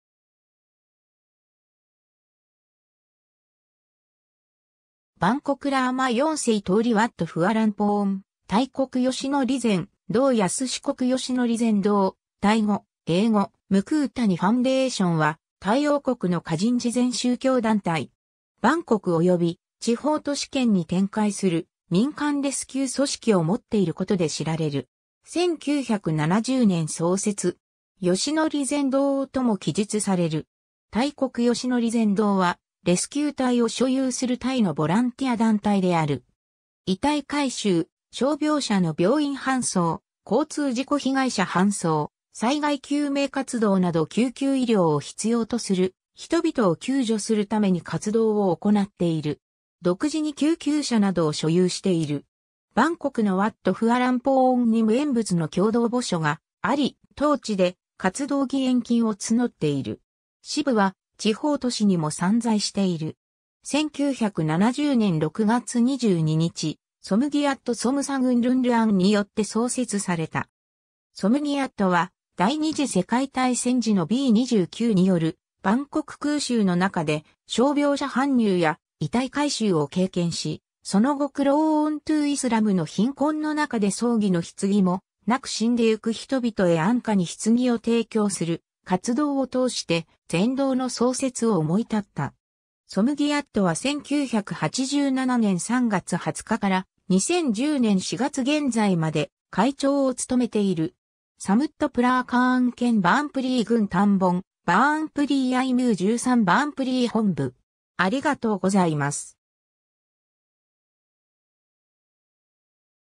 泰国義徳善堂
バンコクラーマ4世通り ワット・フアランポーン 泰國義徳善堂 泰國義徳善堂 (タイ語:มูลนิธิร่วมกตัญญู、英語：Ruamkatanyu Foundation ）は、タイ王国の華人慈善宗教団体（善堂）。
バンコクおよび地方都市圏に展開する民間レスキュー組織をもっていることで知られる。
1970年創設。
「義徳善堂」とも記述される。
 泰國義徳善堂は、レスキュー隊を所有するタイのボランティア団体である。
遺体回収、傷病者の病院搬送、交通事故被害者搬送、災害救命活動など救急医療を必要とする人々を救助するために活動を行っている。
独自に救急車などを所有している。
バンコクのワット・フアランポーンに無縁仏の共同墓所があり、当地で活動義捐金を募っている。
支部は地方都市にも散在している。
 1970年6月22日、ソムギアット・ソムサグンルンルアン（สมเกียรติ สมสกุลรุ่งเรือง）によって創設された。
ソムギアットは第二次世界大戦時のB29によるバンコク空襲の中で傷病者搬入や遺体回収を経験し、その後クローントゥーイスラムの貧困の中で葬儀の棺もなく死んでゆく人びとへ安価に棺を提供する活動を通して、善堂の創設を思い立った。
ソムギアットは 1987年3月20日から、2010年4月現在まで会長を務めている。
 サムットプラーカーン県 バーンプリー郡 タンボン・バーンプリーヤイ　（バーンナー‐トラート道路 12km）ムー13　バーンプリー本部　（มูลนิธิร่วมกตัญญู สำนักงานบางพลี เลขที่19 ม.13 (บางนาตราด กม.12) ต.บางพลีใหญ่ อ.บางพลี จ.สมุทรปราการ）